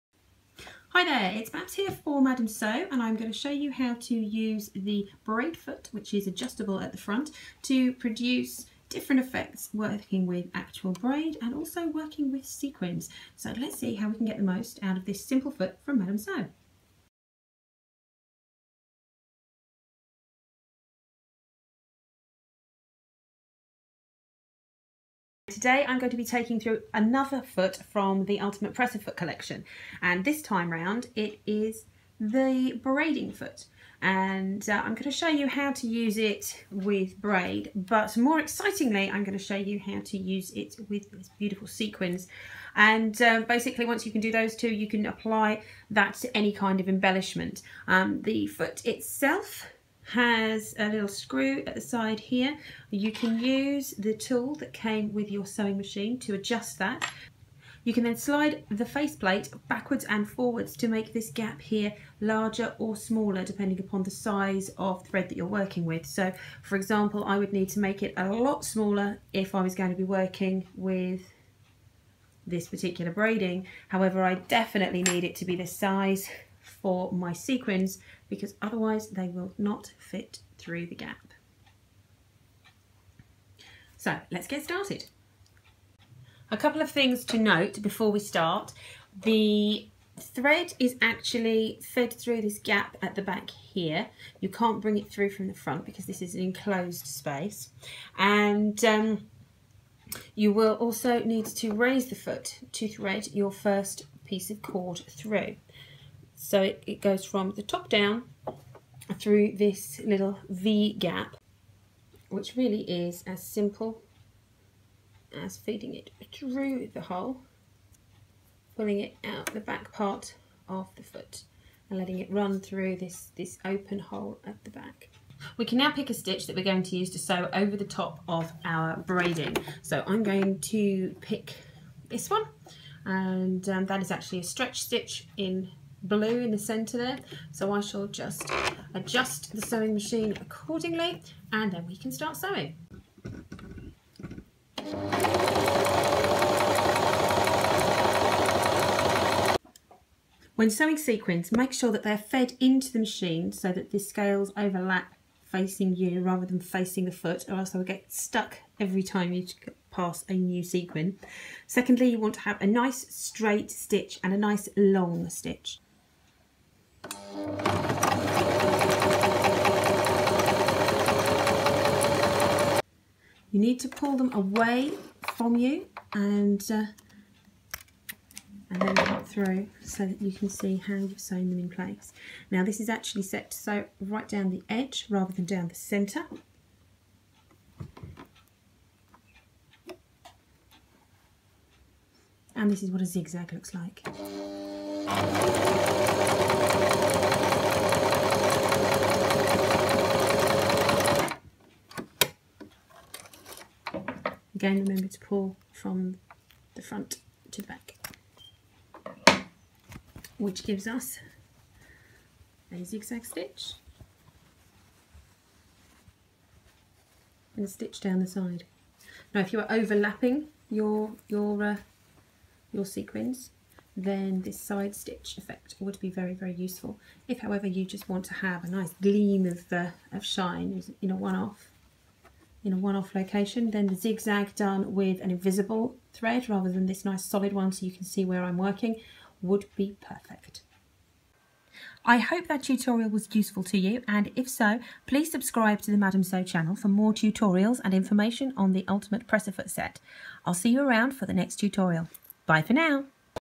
Braid Foot (#9) Tutorial for Madamsew's Ultimate Presser Foot Set
Sew Easy, Sew Effortlesly with Madam Sew.

How to use the Adjustable Braiding foot with a narrow braid and also sequins to make your next project pop and shine!

Jump to the desired section of the video:

00:24 Introduction to the braid foot and how to adjust it
01:26 How to attach a piece of cord to fabric
02:30 Sewing sequins with a straight stitch
03:10 Sewing sequins with a zig-zag stitch